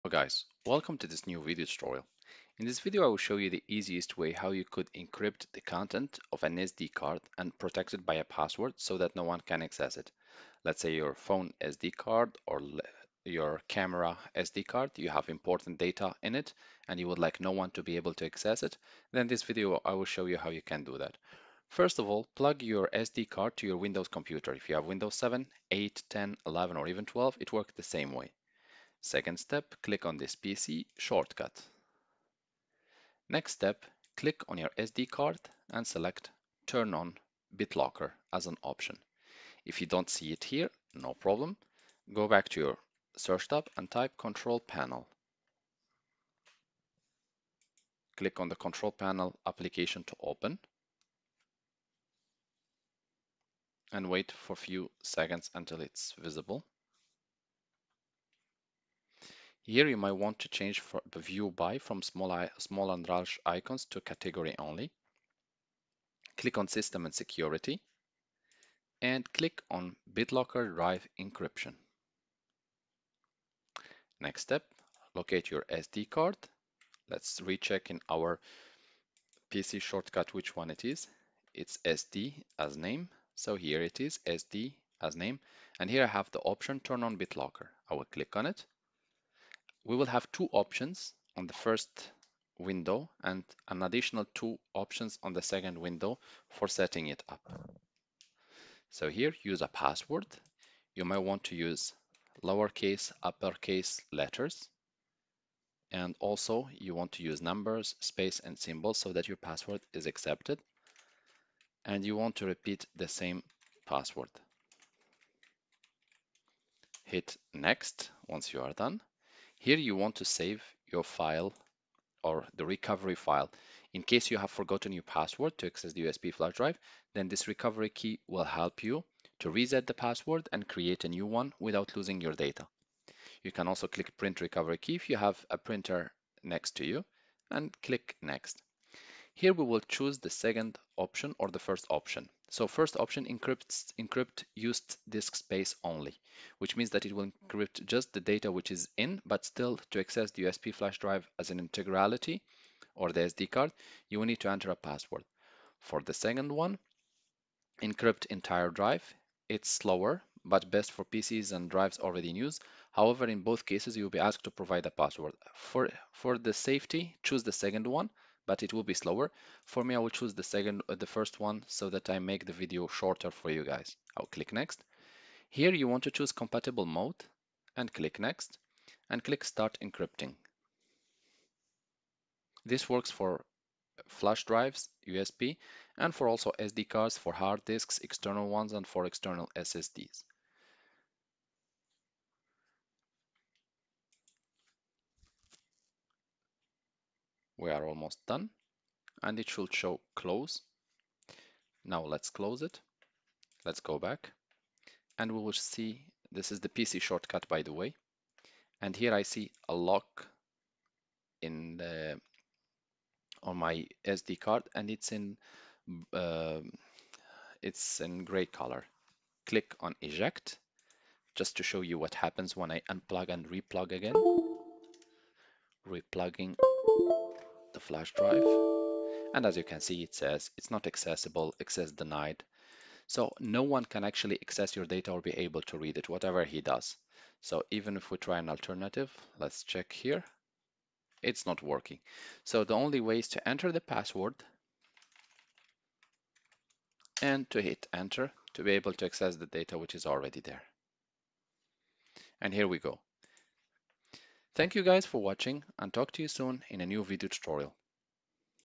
How to Encrypt the Content of an SD Card in Windows!"
Protect your valuable data with our step-by-step tutorial on encrypting the contents of an SD card in Windows! Whether it's personal photos, important documents, or sensitive files, we'll guide you through the process of adding an extra layer of security to your SD card. Watch now and empower yourself with the essential skills to keep your digital life safe and sound. Enhance your data protection journey with this must-watch tutorial! 🔐📸